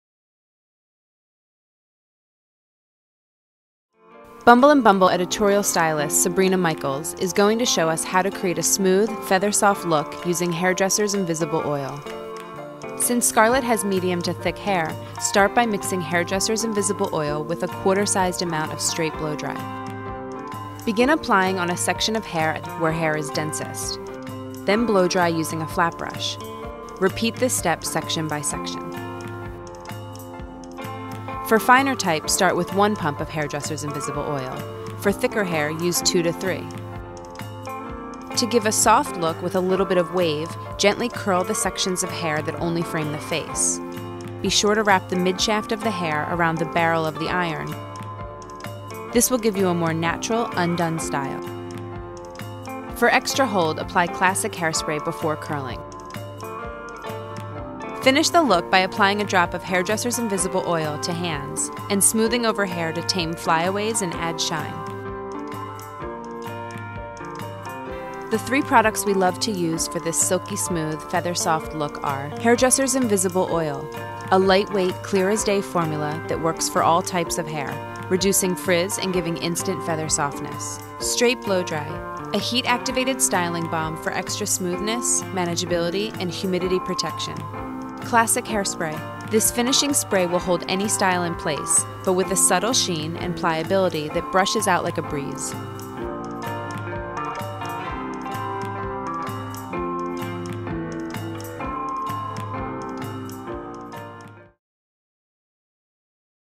MECCA Masterclass: Bumble and Bumble Hairdresser's Invisible Oil
Bumble and Bumble editorial stylist Sabrina Michals shows you how to create a smooth, feather-soft look using Hairdresser's Invisible Oil

Sabrina begins by mixing Hairdresser's Invisible Oil with Straight Blow Dry and running it through the hair:

Continue by blowdrying the hair with a flat brush.

For a more natural, undone look Sabrina recommends softly curling hair away from your face.

For extra hold, ensure you apply Classic Hairspray before your curl your hair: